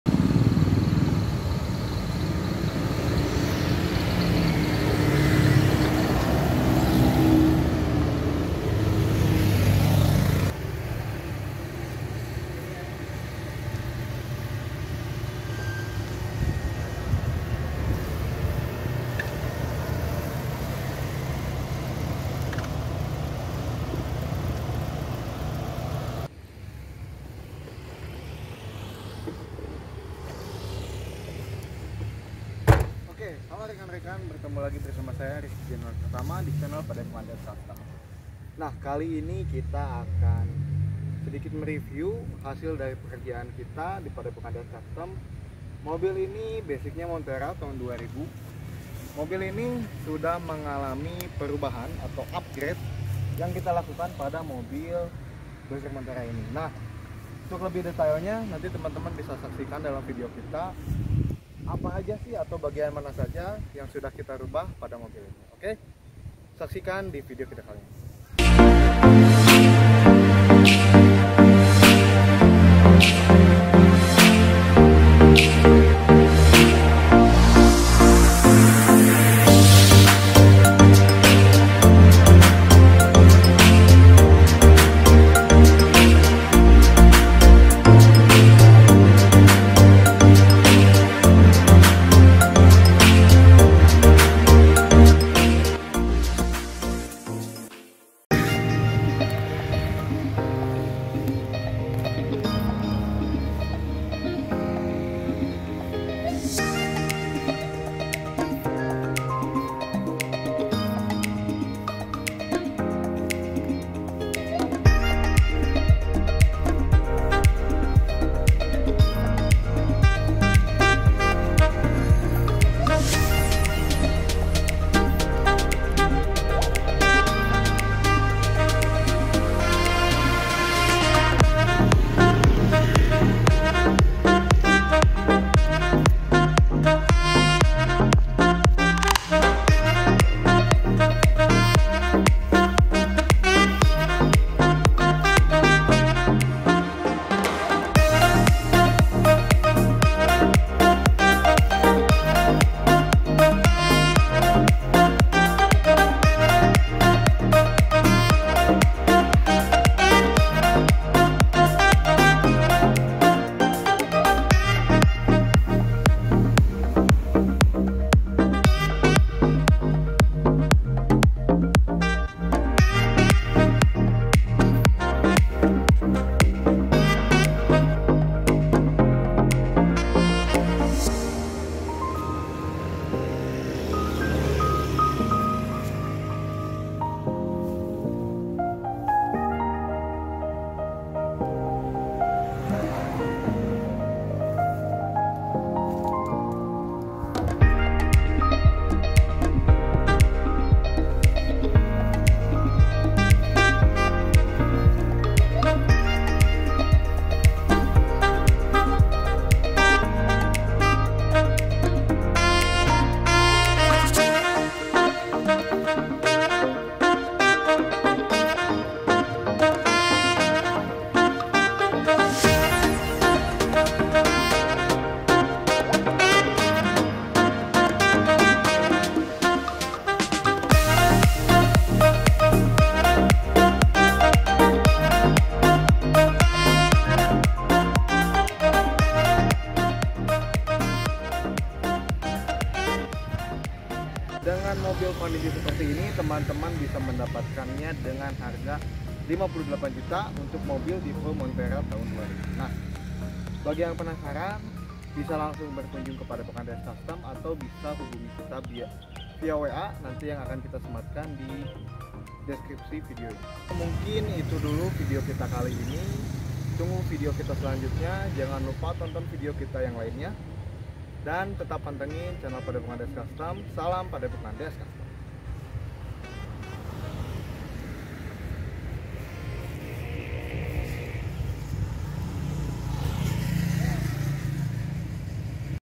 Opel Blazer montera 2000 up grade interior full Dohc/ Dijual 58jt
Opel Blazer montera 200p
interior full Dohc
dortrim chevrolet samba
cat baru
kaki kaki baru
tanduk depan Arb
bemper belakang di lengkapi towing rollbarArb
pajak panjang stnk 2025
harga 58jt nego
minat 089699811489

Untuk konsultasi Restorasi dan Modifikasi hubungi no wa: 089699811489
Alamat Jl. Sumber Sari No.89 kel.Cisaranten kulon kec.Arcamanik
bandung